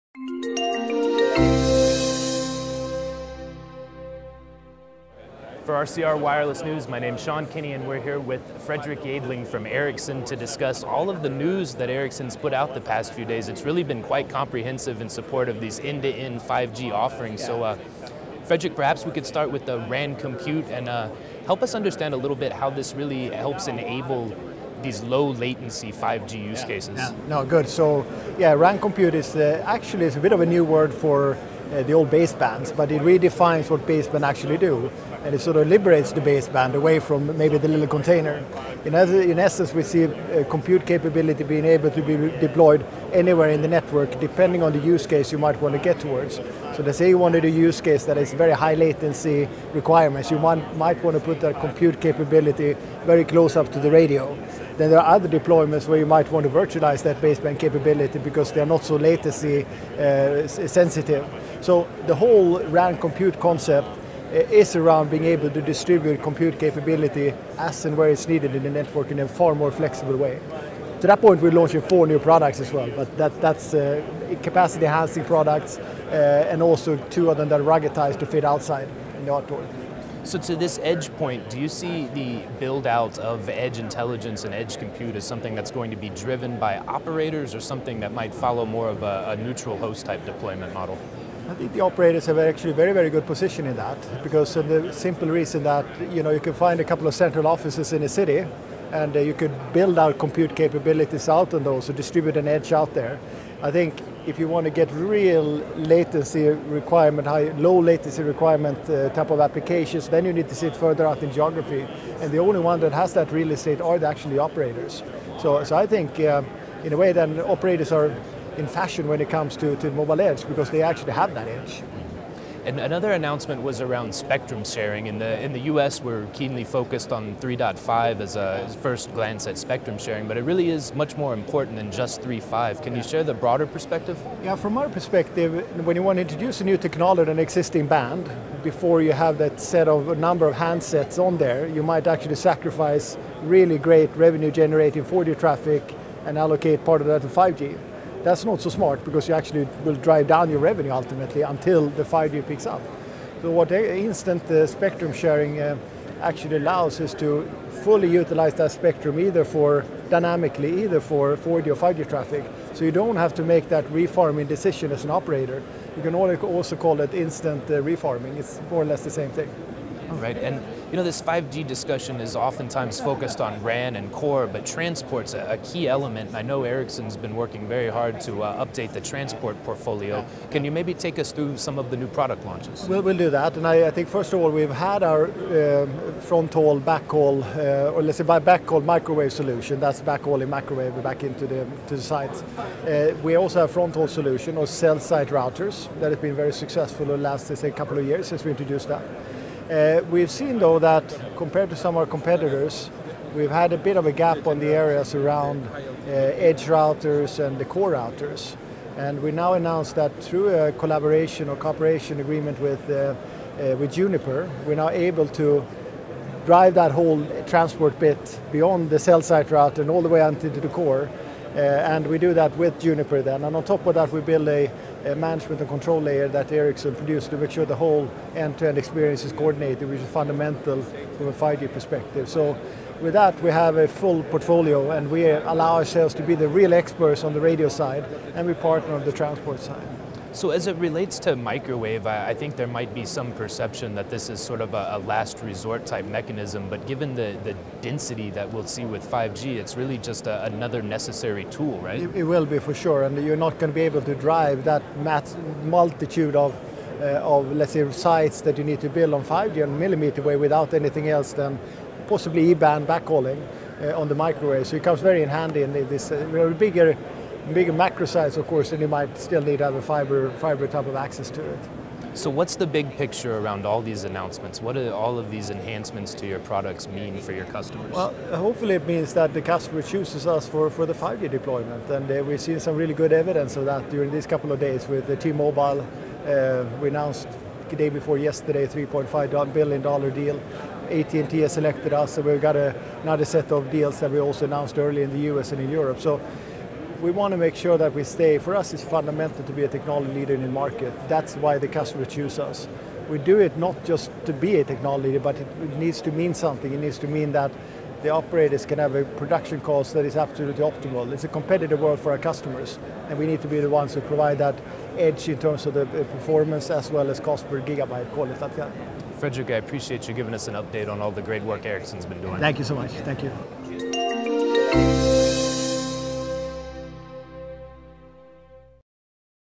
Overview of the latest launches from Ericsson; 5G Transport and Dynamic Spectrum sharing
Fredrik Jejdling, EVP & Head of Networks, Ericsson provides an overview of the company's 5G efforts during Mobile World Congress Americas 2018. Jejdling discusses fronthaul, spectrum sharing, network densification, edge computing and more.